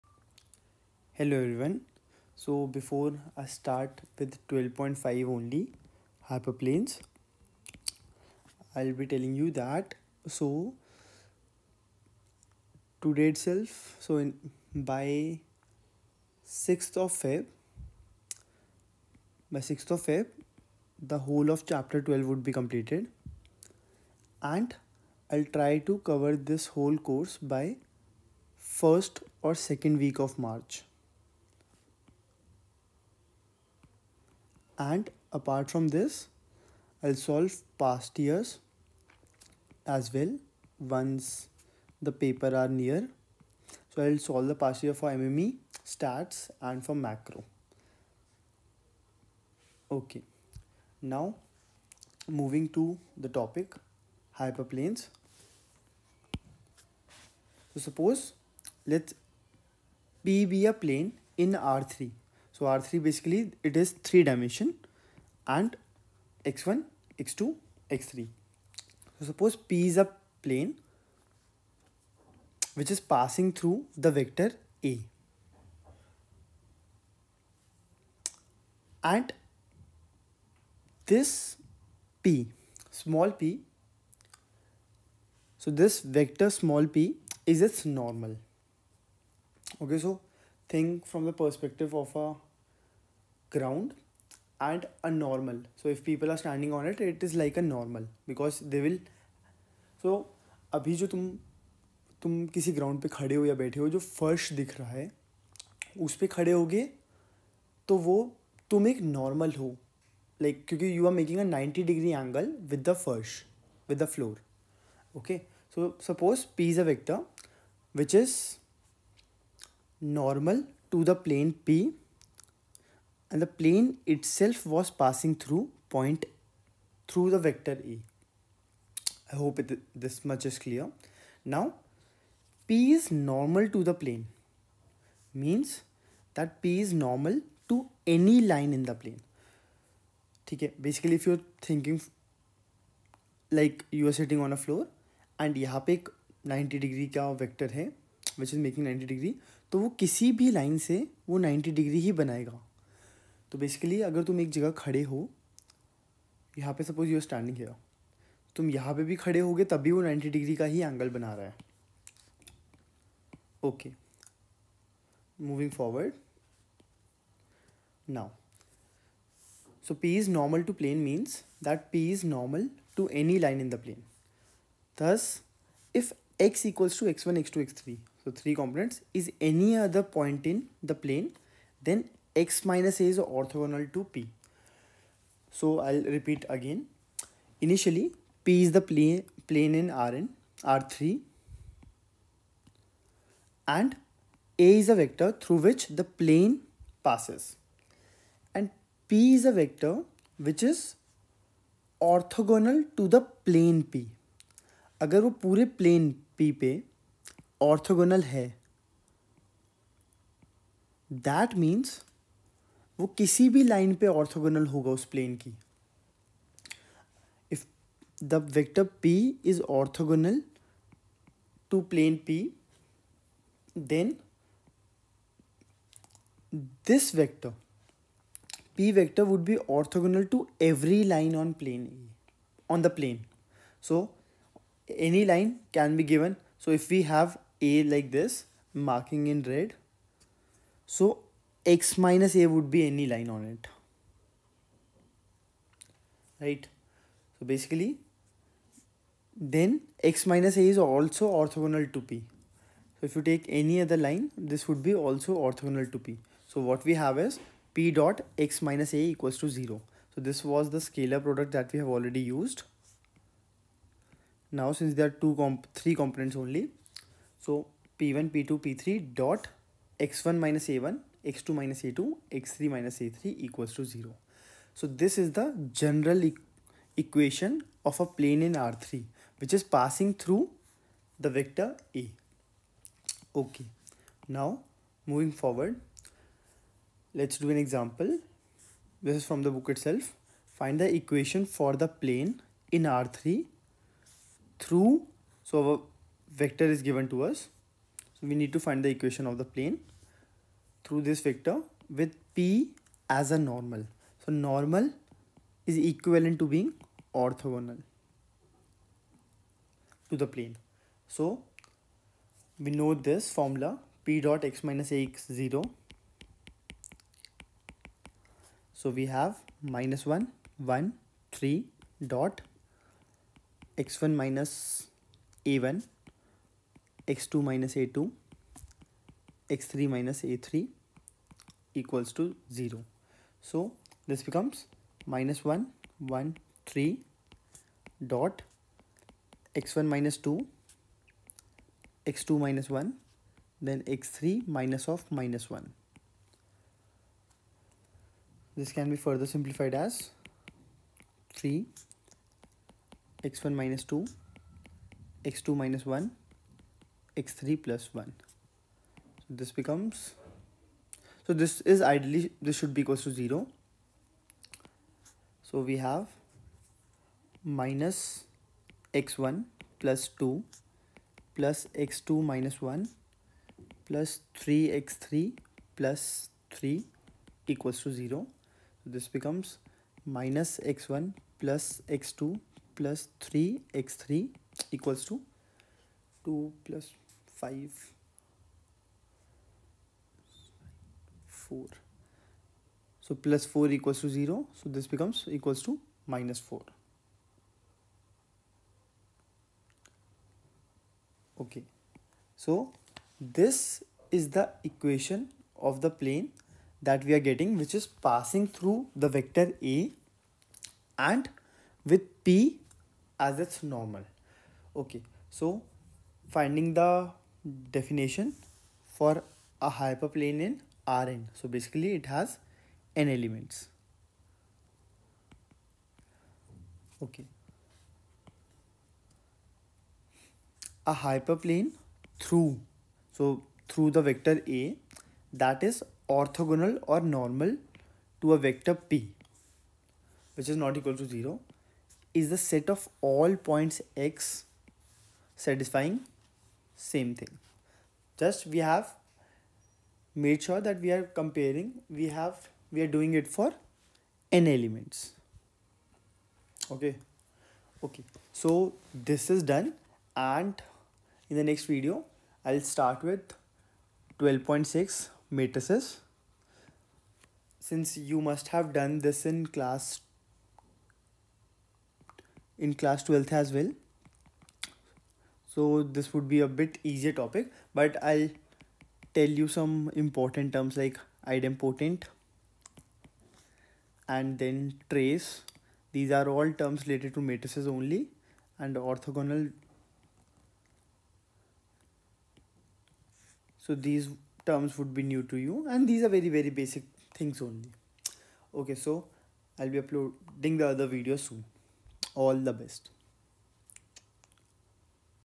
Lecture 7- Hyperplanes
In this video, I have explained the general equation of a Hyperplane(Chapter-12, Exercise-12.5)